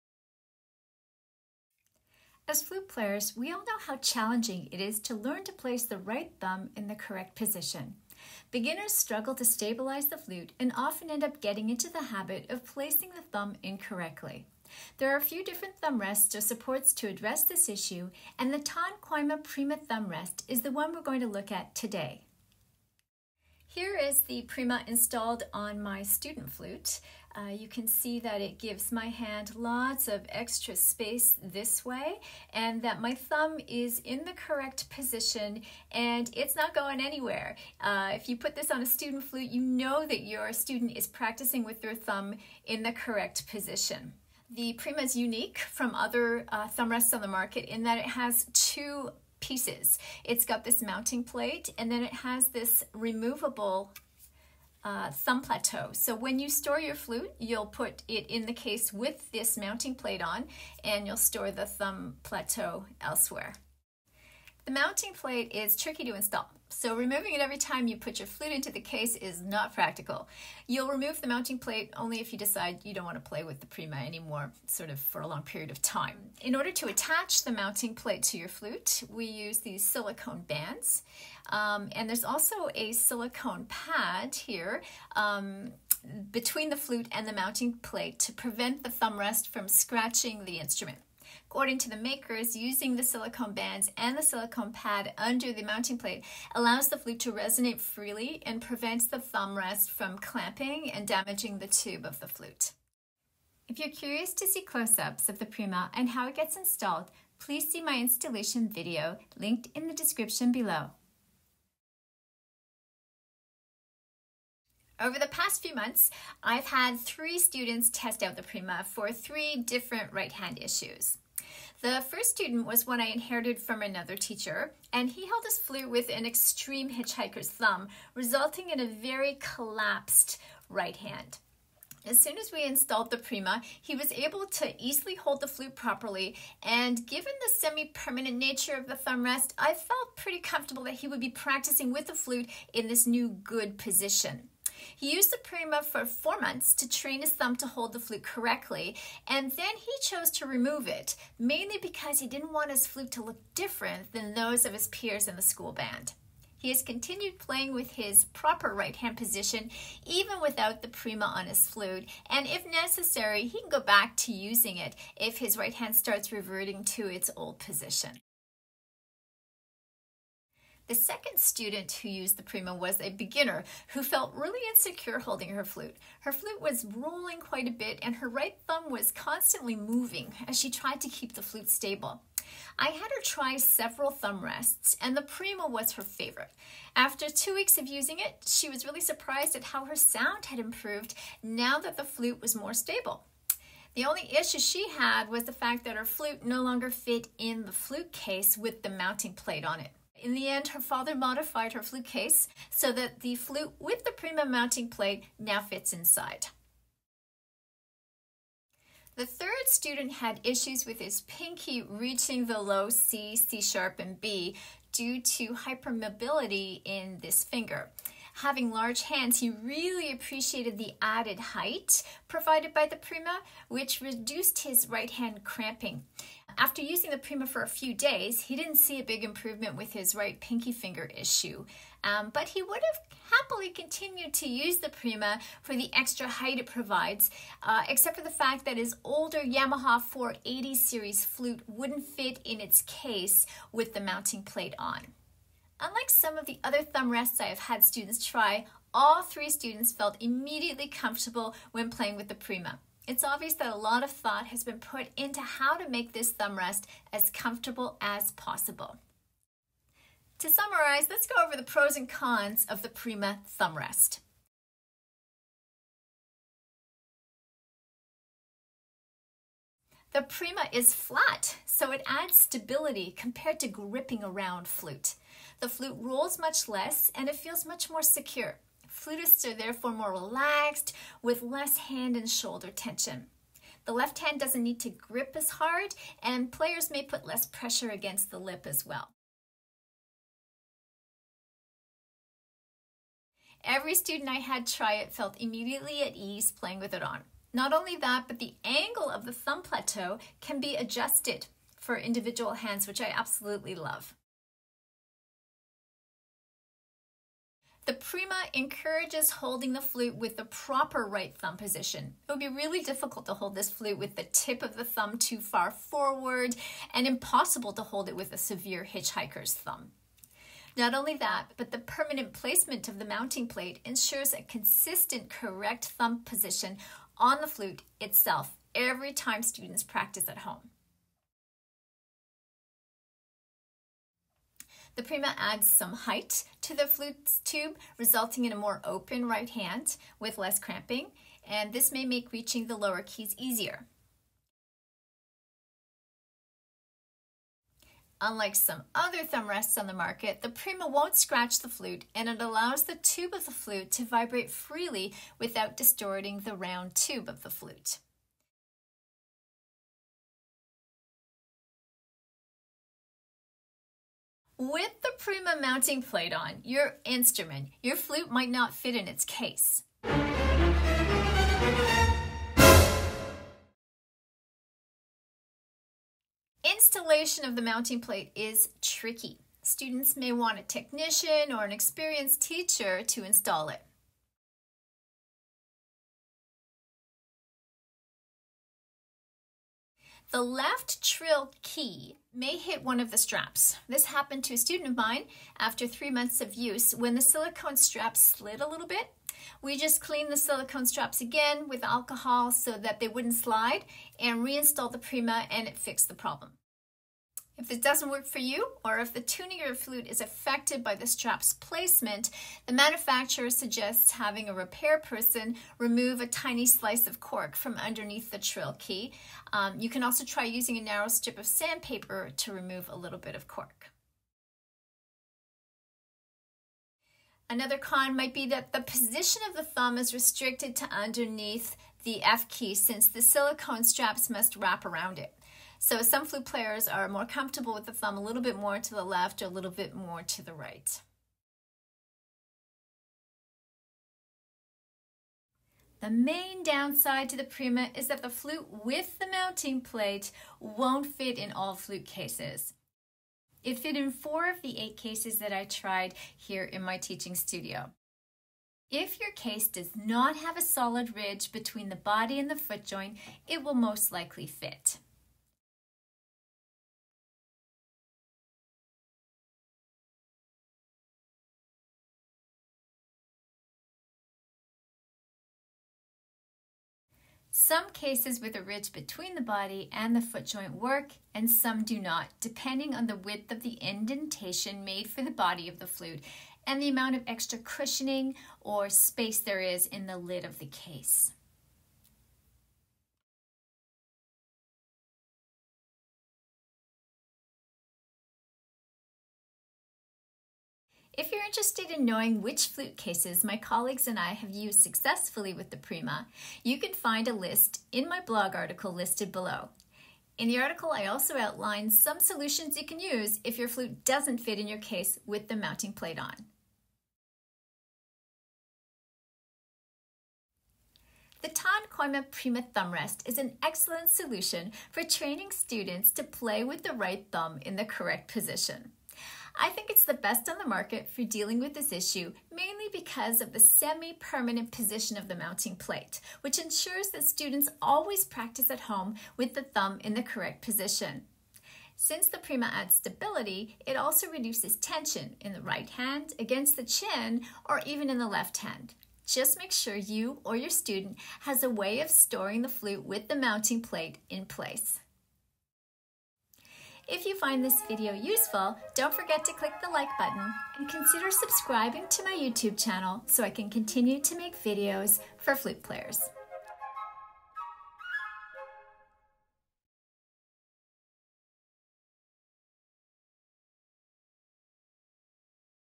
A Review of the Ton Kooiman Prima Thumb Rest for the Flute
In this video, I review the Ton Kooiman Prima Thumb Rest for the Flute.  I’ll show you how I have used it to address right hand issues with three of my flute students and then I’ll go over the pros and cons of this thumb rest. I hope that watching this video will give flute teachers and flute players a good idea as to whether this thumb rest will be useful for them.

If you’d like to see more closeup details of the Prima Thumb Rest, as well as how to install it on the flute, please watch my installation video found here:

You can also read find more details about the Prima in my blog article:

Time code:
00:00 Intro
01:56 Testing the Prima
04:20 The Pros and Cons
07:17 Flute Cases and the Prima
08:28 The Final Verdict